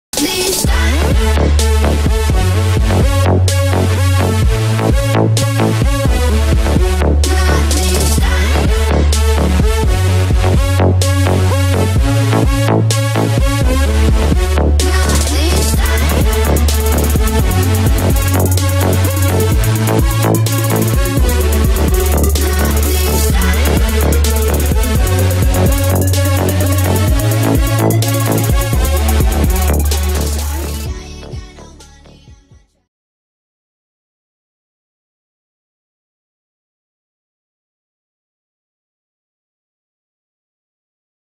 Tremorz & Terrorz | Promo Video with spectrum effect
Tremorz & terrorz promo video. I made that effect on adobe after effect cs6 for my friend...